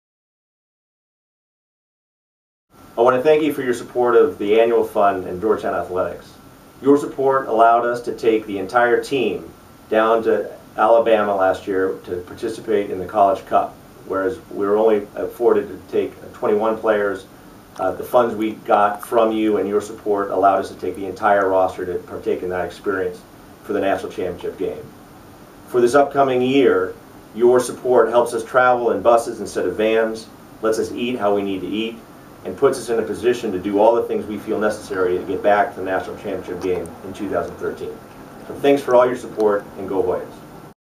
Brian Wiese - Thank You
Head Men's Soccer Coach Brian Wiese thanks donors for their support this past year!